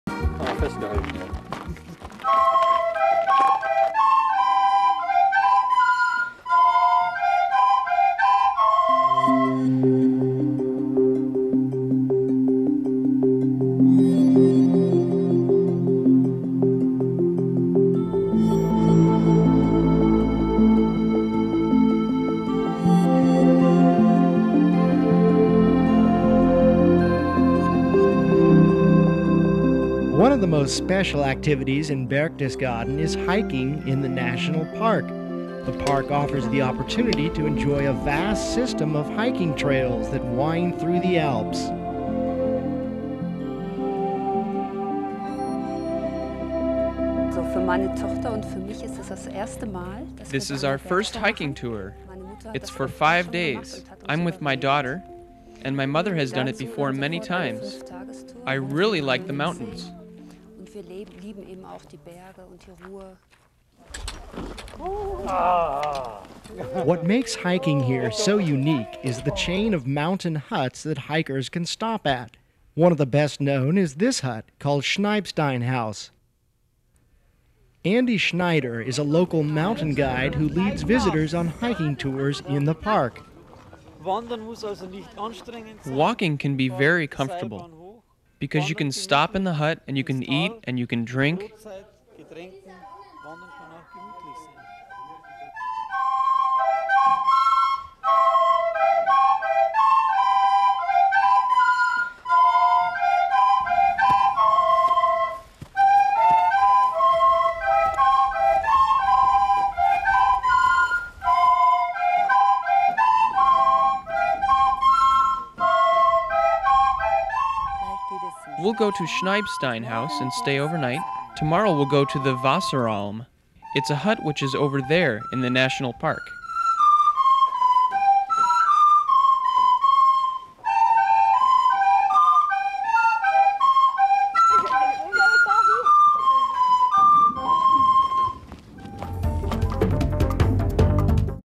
Bavaria: Hiking
Hiking segment with gorgeous scenery, hut to hut. Bavaria in southern Germany is an incredible place for adventure. We'll take you biking, hiking, boating, river rafting, canyoning, through the salt mines, parasailing. Tons of adventures.